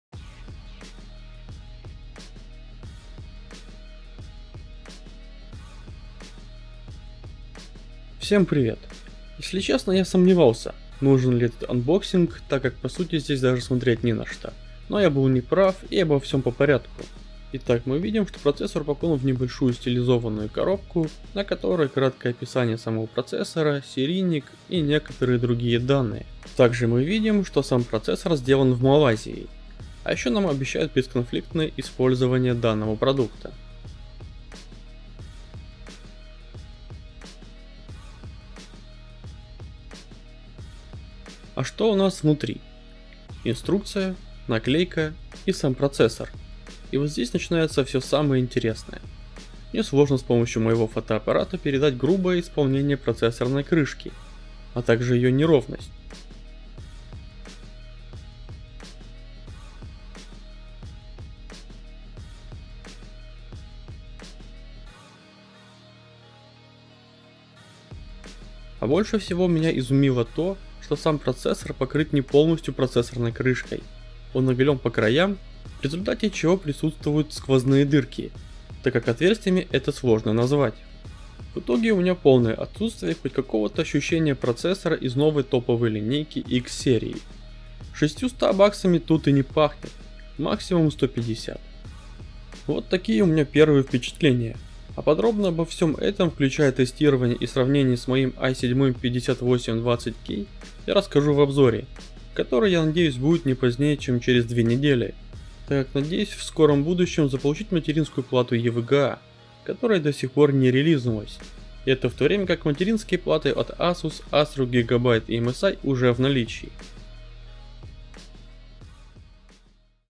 i7-7820X - распаковка/unboxing - анонс обзора и тестирования
Распаковка (unboxing) процессора X-серии Intel i7-7820X (LGA2066), под материнские платы на чипсете X299/